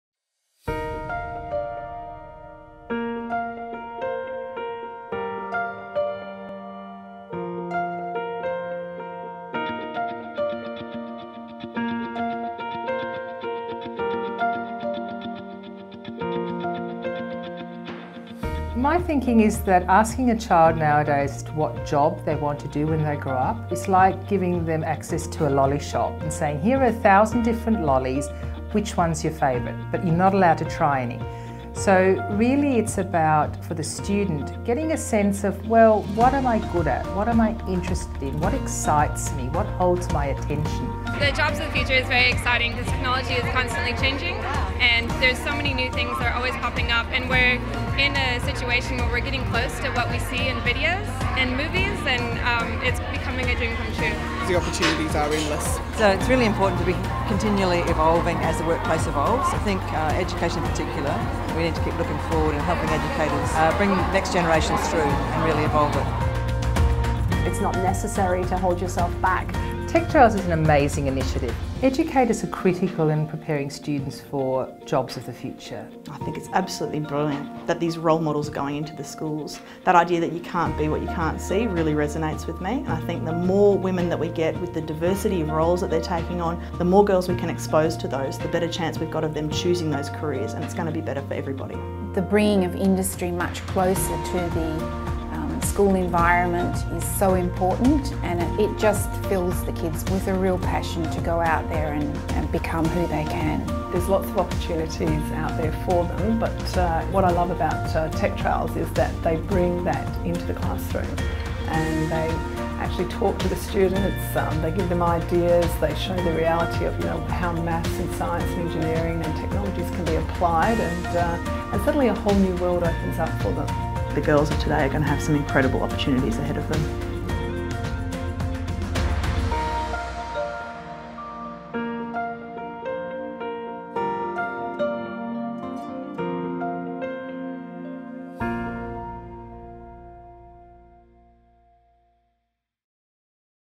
Blazing a path to exciting STEM careers
Future of work. How WiTWA is inspiring and supporting young people make decisions about jobs for their future. A visual celebration of WiTWA's community engagement and signature schools program, techtrails.